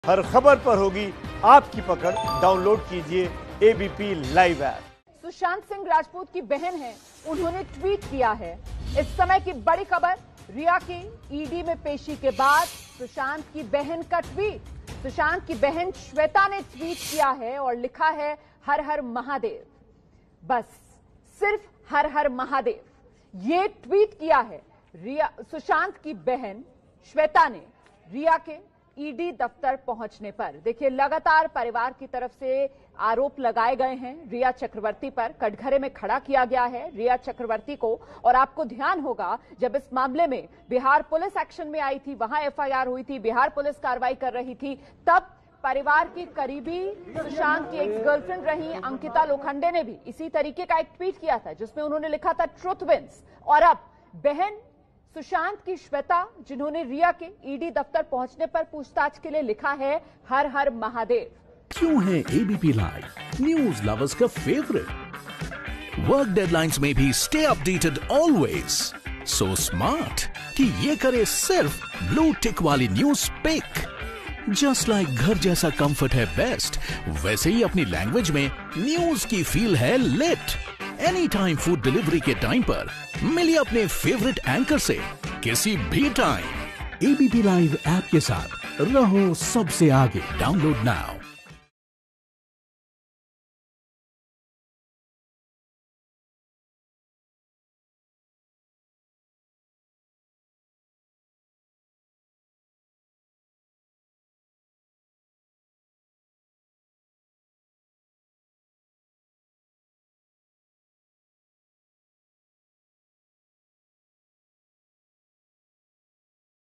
Rhea Chakraborty की ED के सामने पेशी से पहले सुशांत की बहन का ट्वीट, "हर हर महादेव" | ABP News Hindi
Rhea Chakraborty के वकील ने ईडी से गुहार की थी कि जब तक  Supreme Court में मामले की सुनवाई चल रही है, तब तक इस मामले में ईडी उनसे पूछताछ नहीं करें. मगर ईडी ने उनके एप्लिकेशन को रिजेक्ट कर दिया है.